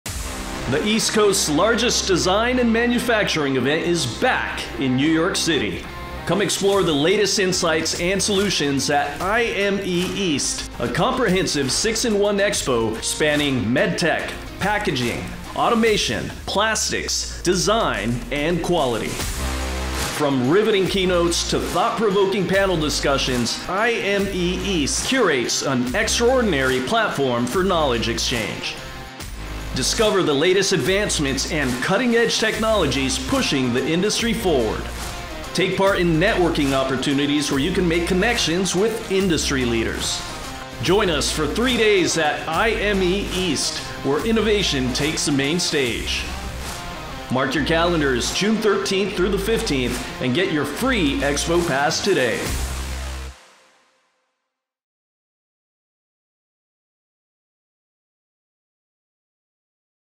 2023 IME East Promo
With six expos under one roof, the East Coast’s leading engineers and innovators will be at ATX East, D&M East, MD&M East, Plastec East, EastPack, and Quality Expo. Join the advanced manufacturing community to solve problems, source solutions, and move manufacturing forward.